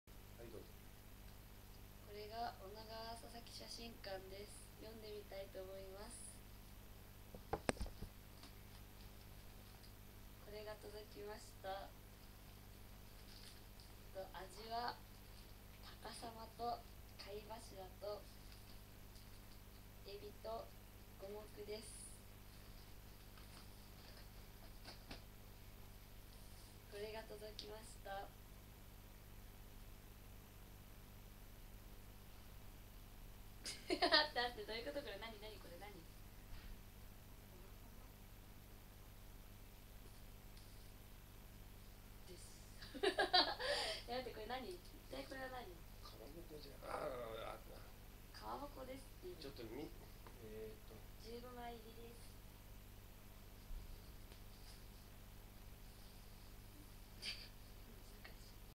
【開いたお店の商品を買ってこそ復興支援】鈴木麻弓著 女川佐々木写真館―2011年3月11日、その日から。株式会社高政 宮城県牡鹿郡女川町のあげかま12枚とおなかま:15枚購入。
鈴木麻弓著 女川佐々木写真館―2011年3月11日、その日から。

株式会社高政　宮城県牡鹿郡女川町のあげかま12枚
（たかまさあげ・かいばしら・ごもく・えびの4種類)とおなかま:15枚購入しました。

女川# 女川佐々木写真館#高政#鈴木麻弓#たかや旅行#たかや#365日Vlogしよう